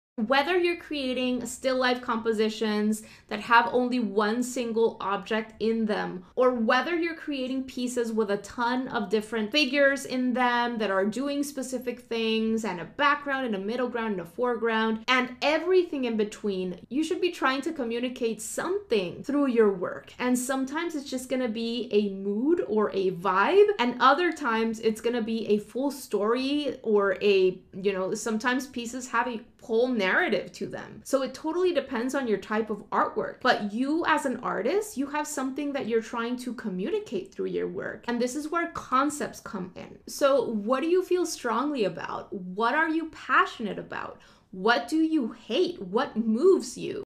What does your art communicate?
All art communicates a message, mood, idea, or story/narrative.
As artists, we need to constantly reflect on what we're trying to communicate through our drawings/paintings and practice self-evaluation to continue improving.